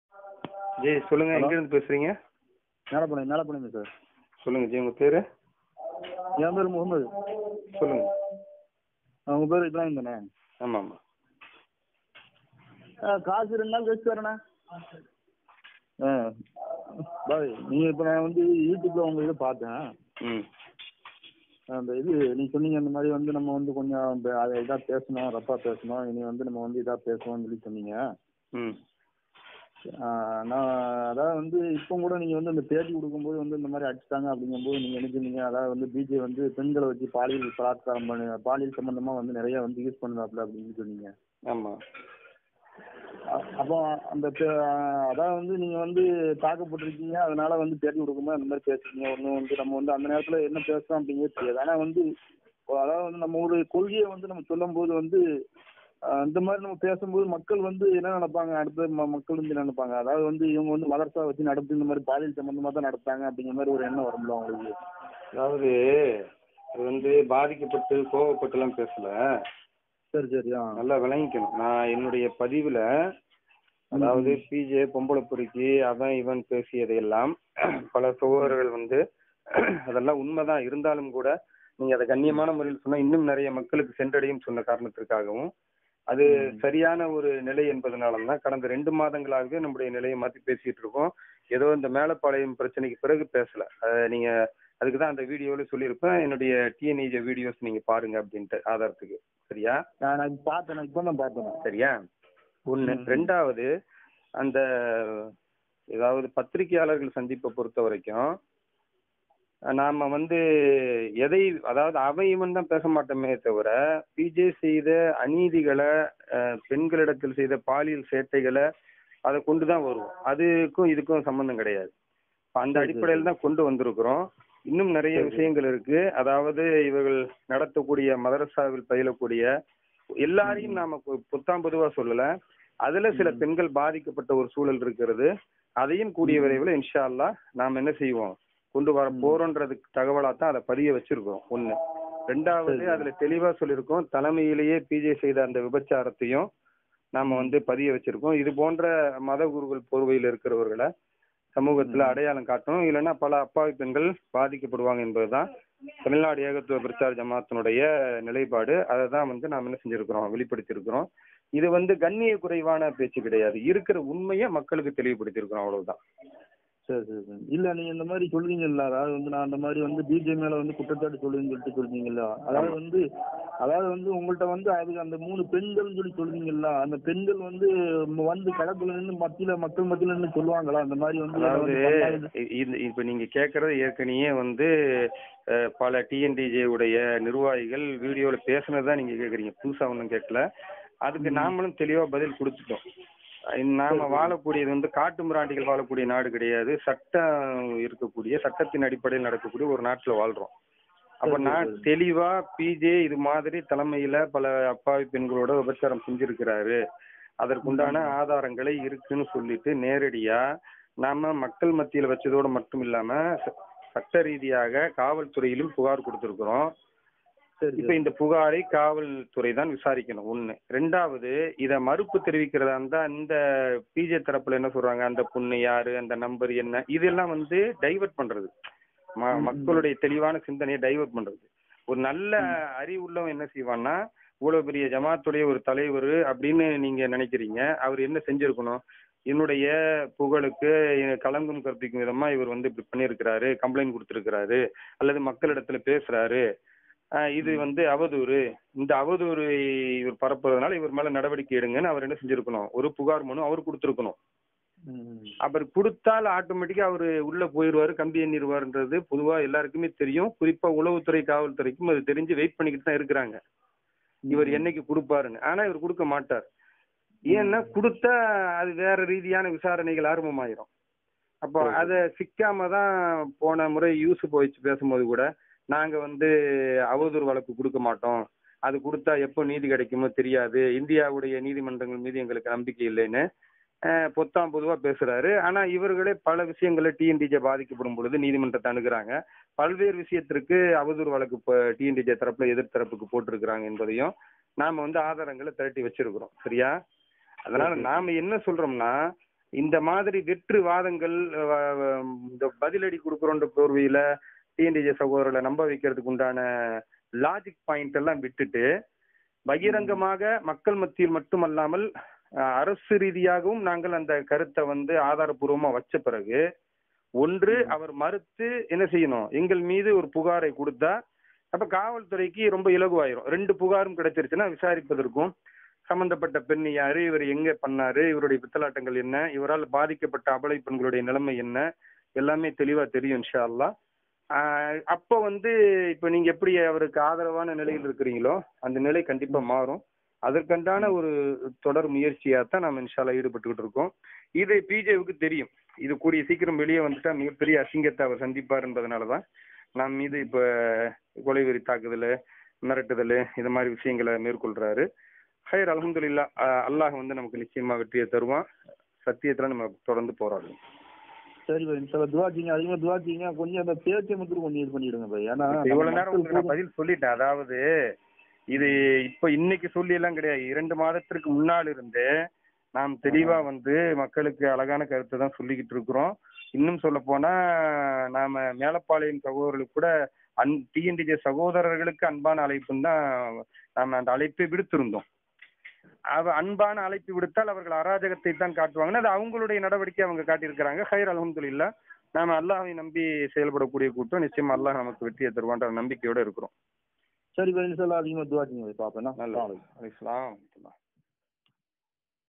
TNTJ மதரஸாவில் தவறு நடக்கிறதா?
உரை:
வேலூர் M. இப்ராஹிம்,M(Acu), BA,BL. அவர்கள்,
மாநில தலைவர்,
தமிழ்நாடு ஏகத்துவ பிரச்சார ஜமாஅத் (TNEJ).